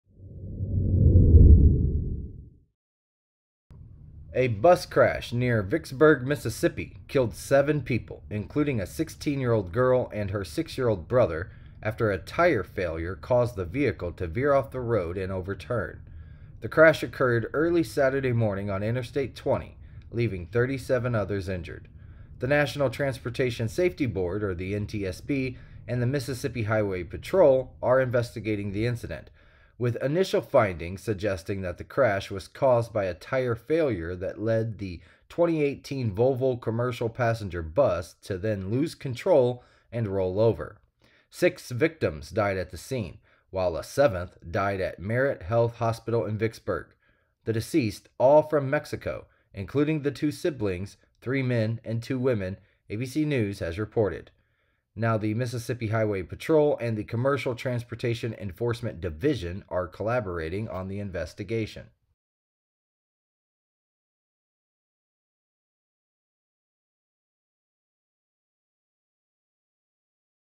Tire failure suspected in Mississippi bus crash that killed 7 and injured 37
A bus crash near Vicksburg, Mississippi, killed seven people, including a 16-year-old girl and her 6-year-old brother, after a tire failure caused the vehicle to veer off the road and overturn.

The crash occurred early Saturday morning on Interstate 20, leaving 37 others injured.

The National Transportation Safety Board (NTSB) and Mississippi Highway Patrol (MHP) are investigating the incident, with initial findings suggesting the crash was caused by a tire failure that led the 2018 Volvo commercial passenger bus to lose control and roll over.

Six victims died at the scene, while a seventh died at Merit Health Hospital in Vicksburg.

The deceased, all from Mexico, included the two siblings, three men, and two women, ABC News has reported.